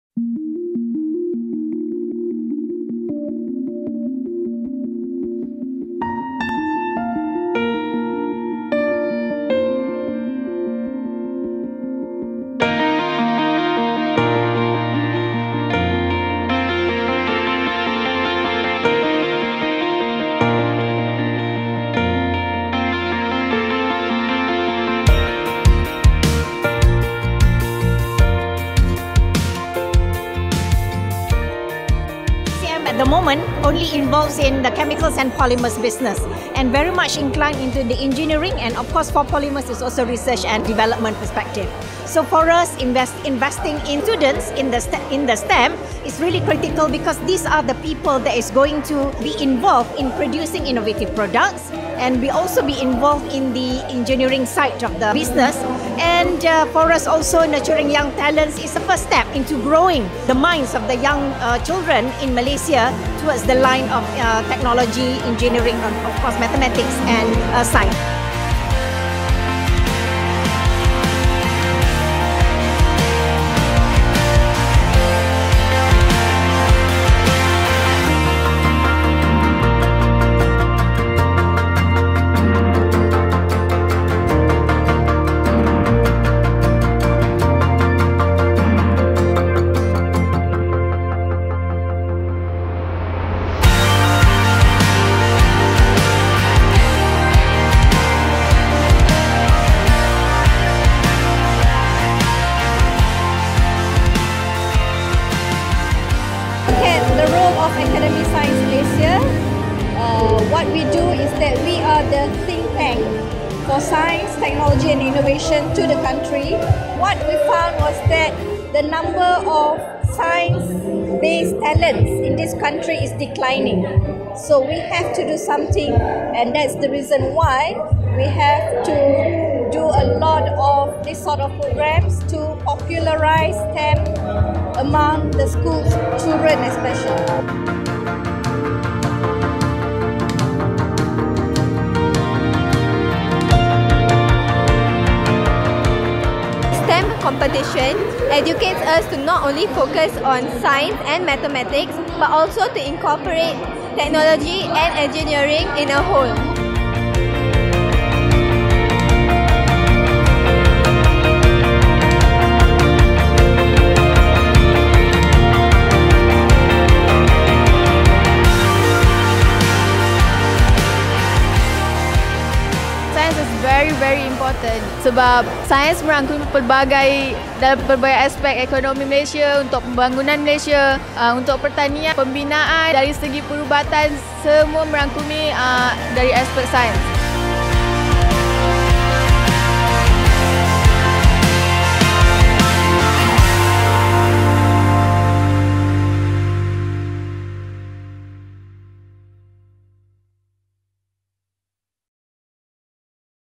CCM STEM UP CHALLENGE 2018
Chemical Company Of Malaysia Berhad (CCM) collaborated with Academy of Sciences Malaysia (ASM) to organise the inaugural CCM STEM Up Challenge 2018 in Batu Pahat, Johor in April.

The competition aims to nurture secondary school students’ interest in Science, Technology, Engineering and Mathematics (STEM) and shape them to become future innovators of Malaysia. It was opened to students in Forms Four and Five in the science stream or their schools’ science club in the Batu Pahat district.

SMK (P) Temenggong Ibrahim emerged as champions of the challenge, while SMK Tinggi Batu Pahat and SMK Dato Seth took second and third place respectively.